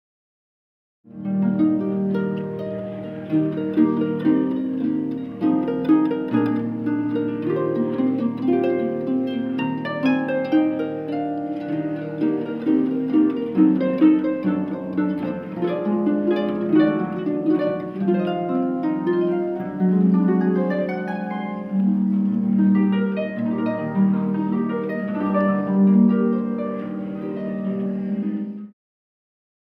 Barbara Lepke playing harp at Skyline Talent Showcase
Barbara Lepke performing at the Skyline Talent & Events showcase, Jan. 2010